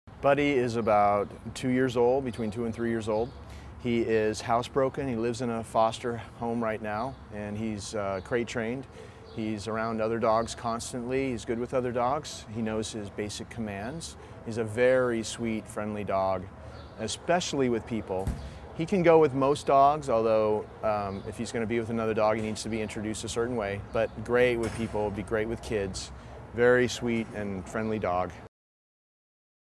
ADOPTED! Buddy - Australian Shepherd / Bernese Mountain Dog Mix - for Adoption
Buddy is a supersweet boy. He's very friendly with everyone and gets along with most dogs, if introduced properly. Buddy is cratetrained and housetrained. He loves to get attention from everyone and isn't shy about cuddling up by your side